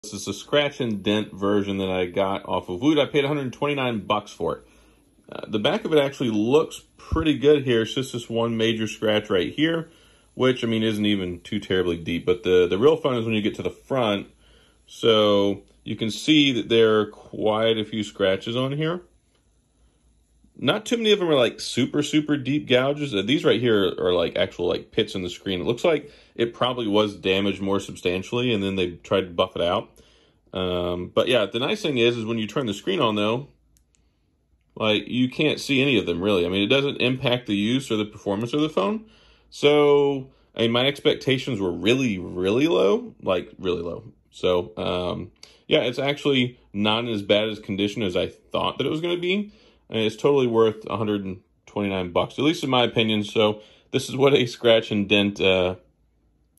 Scratch and Dent Google Pixel 3 XL... Why The Heck Not!
This is a YouTube short about the google pixel 3 XL that I bought off of Woot for $129. Mostly I was just curious how bad a scratch and dent Pixel phone looked so I said screw it and bought it... I’m not disappointed.